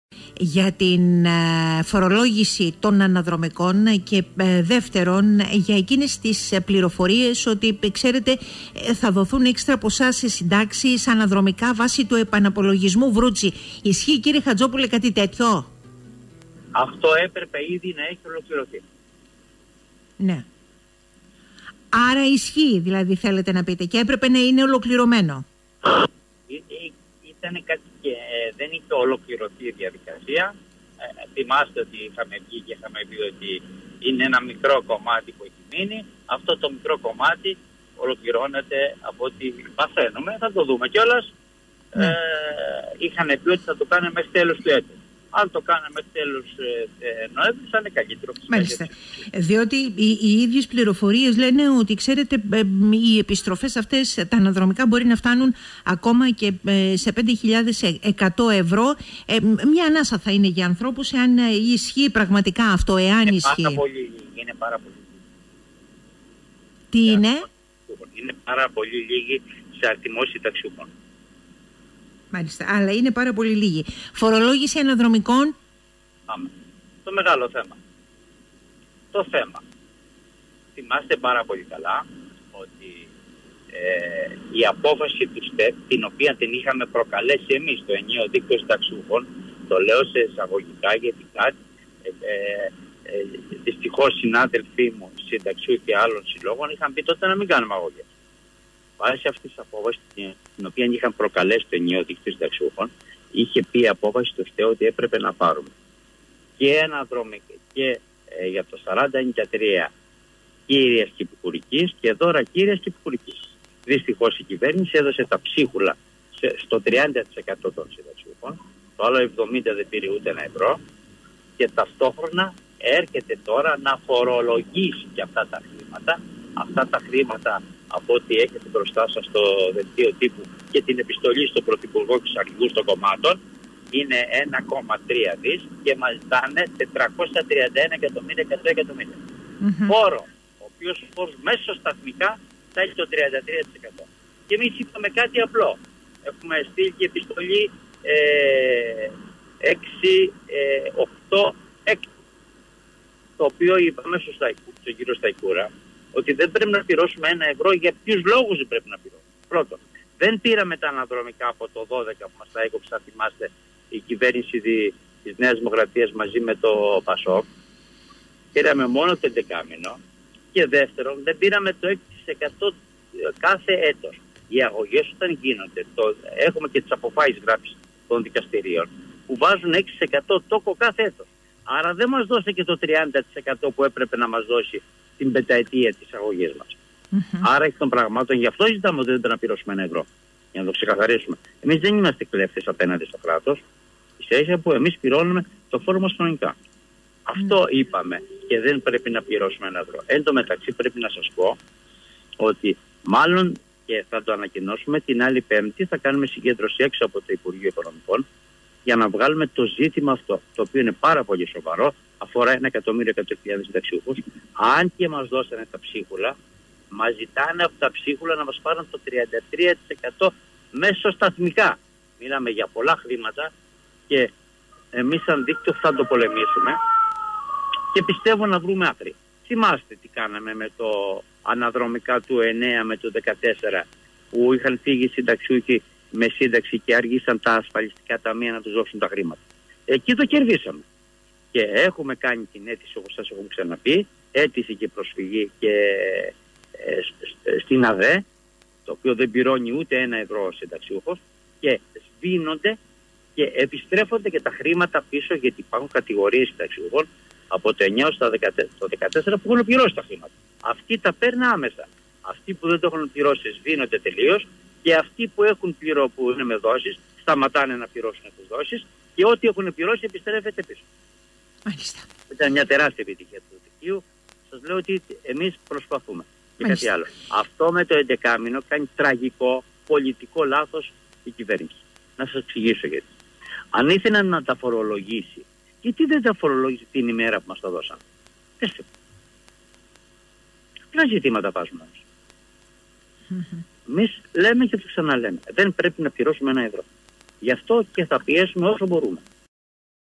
Υπ’ ατμό οι συνταξιούχοι για τη φορολόγηση των αναδρομικών
Ν. Χατζόπουλος στον 98.4: Επιτέλους να μας ακούσουν πρωθυπουργός και Πολιτικοί Αρχηγοί
Νέες κινητοποιήσεις των συνταξιούχων την εβδομάδα 22 με 26 Νοεμβρίου με αίτημα τη μη φορολόγηση των αναδρομικών τους , το οποίο γνωρίζει το σύνολο της πολιτικής ηγεσίας της χώρας όπως ο Πρόεδρος του Ενιαίου Δικτύου λέει στον 98.4. Πιθανότατά  την Πέμπτη 25 του μήνα να πραγματοποιήσουν νέες συγκεντρώσεις.
Αναφορικά με τις πληροφορίες περί νέας καταβολής αναδρομικών με βάση το νόμο Βρούτση, ο Νίκος Χατζόπουλος επιβιώνει ότι αυτή θα γίνει καθώς μέχρι τέλος του χρόνου πρέπει να έχει ολοκληρωθεί.